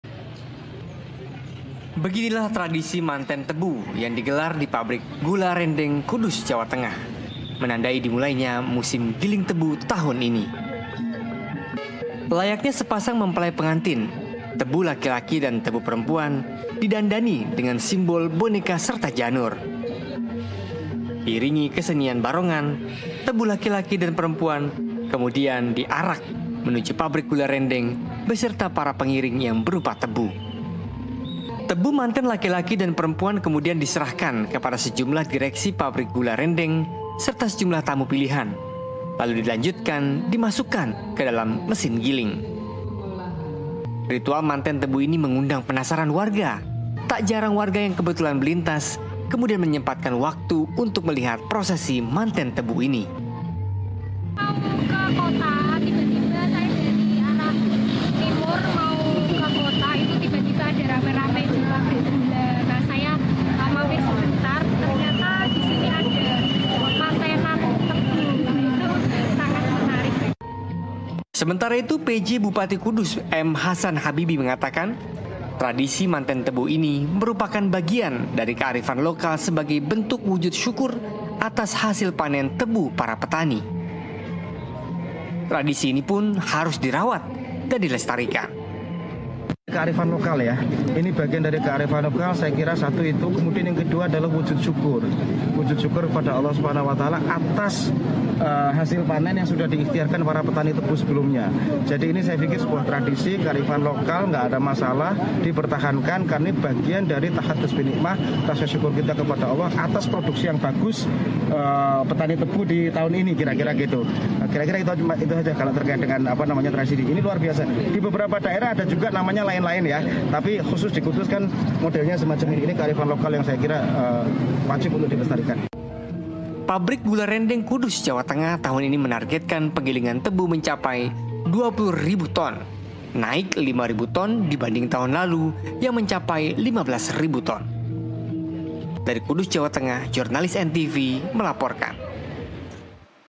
Mengenal Tradisi Unik, Manten Tebu dari Pabrik Gula Rendeng di Kudus | NTV MORNING
Tradisi Manten Tebu yakni menikahkan dua batang tebu sebelum masuk ke penggilangan, sebagai pertanda mengawali musim giling tebu yang bertujuan untuk memohon keselamatan, serta rasa syukur kepada Tuhan Yang Maha Esa.